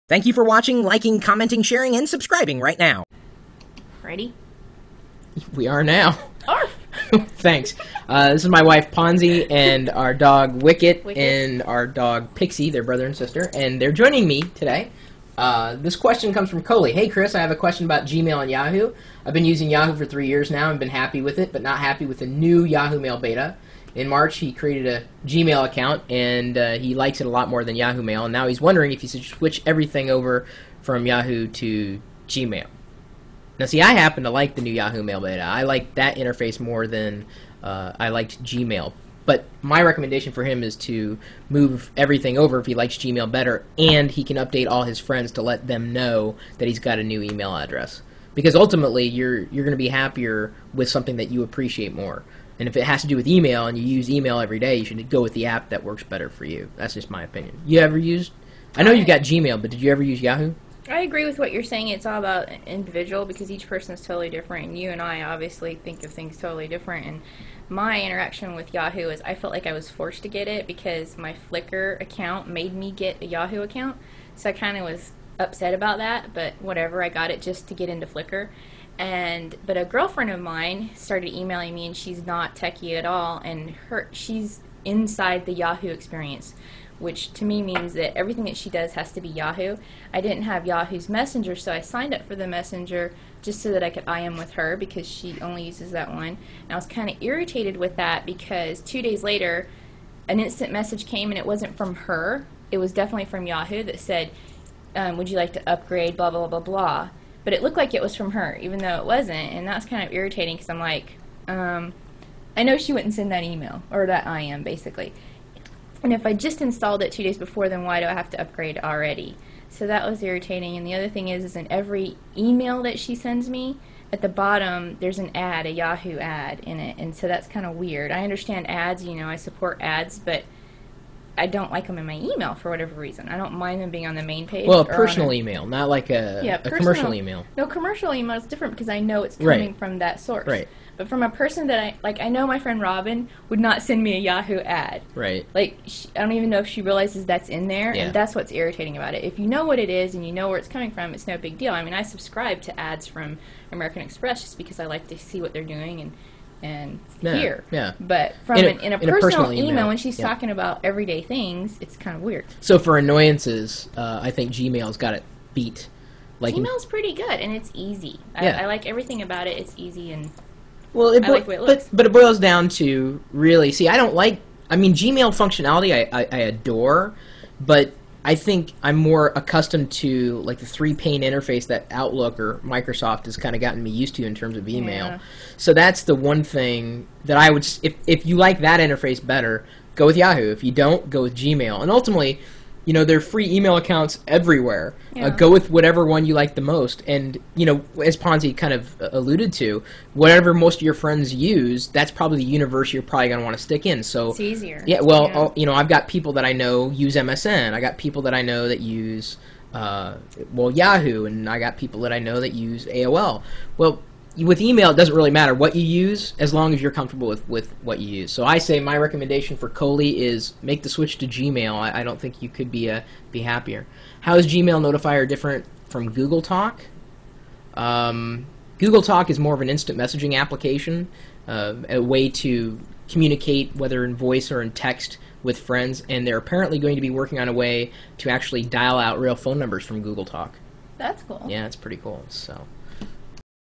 Yahoo! vs gMail
/ - Kole isn't happy with Yahoo! Mail and he likes gMail better. Now he wants to know is if he should move everything over to gMail.